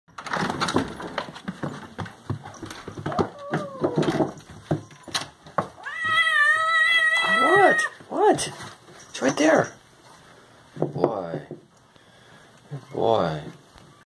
Rowdy on Christmas Morning
Rowdy just couldn't wait to see what Santa had brought him this morning! (Actually, this happens every morning and eve when he gets his wet food treats!)